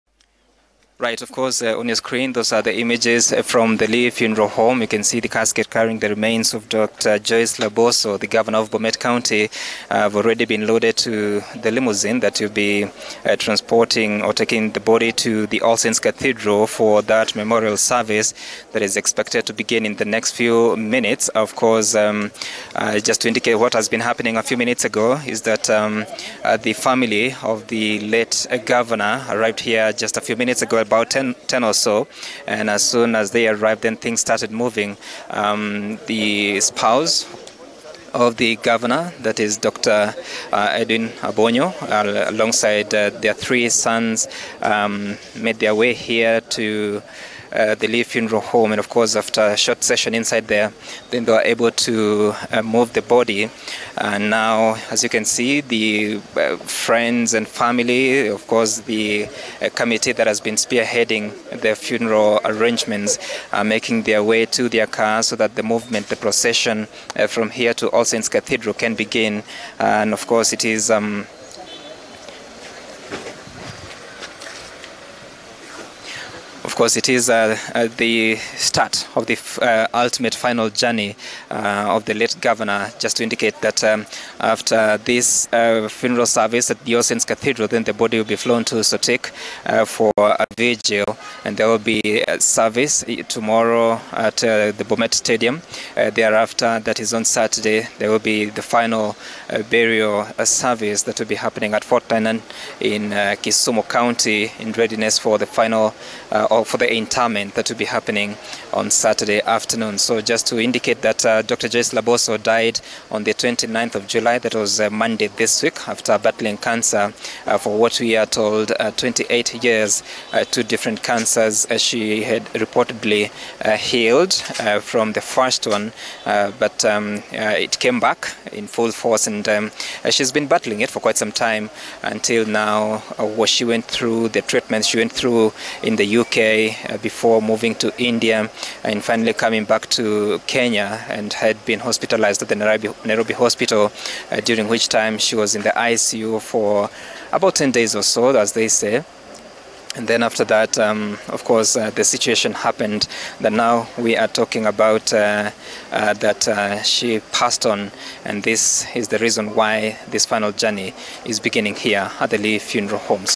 Late Governor Laboso’s body removed from Lee Funeral Home for Requiem Mass
Requiem Mass of the late Bomet County governor is being held at All Saints Cathedral Nairobi.